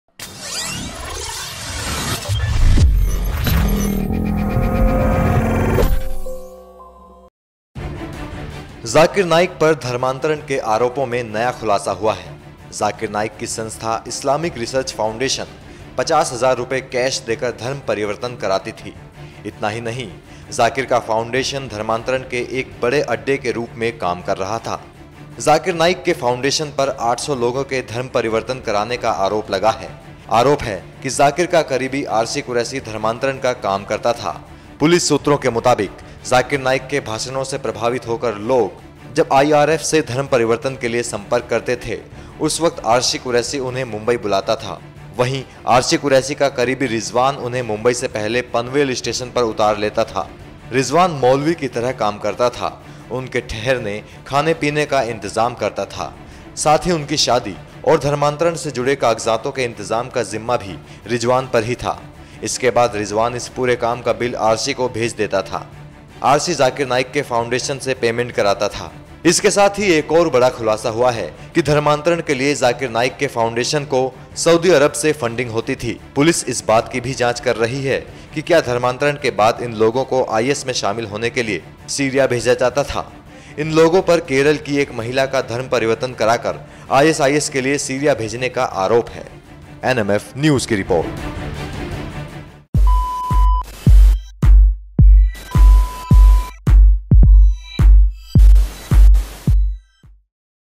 50,000 में धर्म परिवर्तन करवा रहा था Zakir naik का foundation!
The arrest and the subsequent interrogation of two persons suspected to be aides of Dr Zakir Naik has revealed the scale of the conversions (to Islam) that took place. Currently the police are scanning at least 5,700 conversions that have taken place in the past five years. Out of this the police suspect that at least 800 were carried out allegedly by Naik's aides. Further it has also been learnt that those who converted were paid Rs 50,000 each.